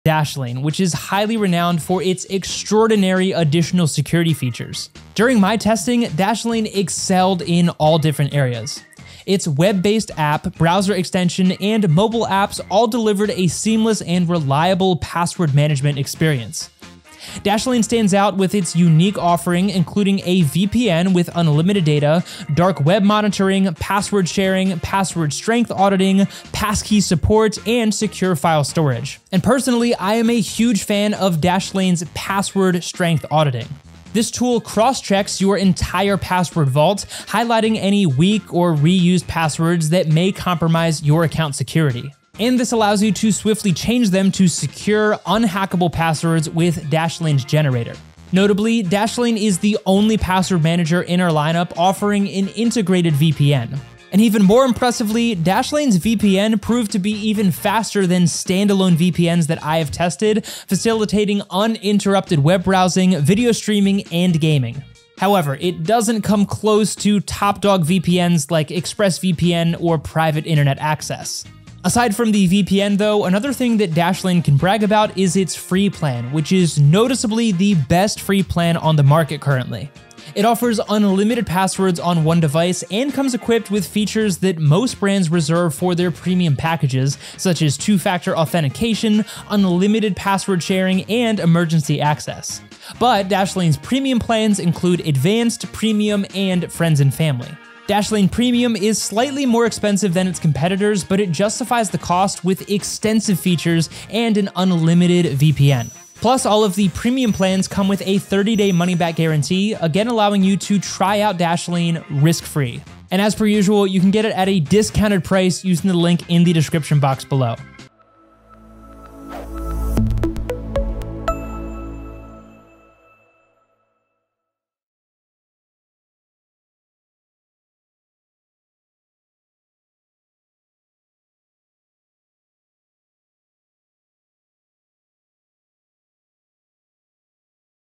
Dashlane Review 2026: Unique Features and Unhackable Passwords🔑Quick 1min Review⏱️
👉Dashlane Discount: 💥

Join us for a quick 1-minute review of Dashlane, the password manager that stands out with its unique features and enhanced security. Dashlane offers an integrated VPN, dark web monitoring, password sharing, password strength auditing, passkey support, and secure file storage. With an intuitive interface and seamless performance, Dashlane ensures a reliable password management experience. Discover how Dashlane keeps your passwords secure and unlocks the convenience of password management in this quick review.

👉To get more in depth info about Dashlane,

00:00 Dashlane Features
00:29 Dashlane Password Strength Auditing
00:48 Dashlane VPN
01:13 Dashlane Plans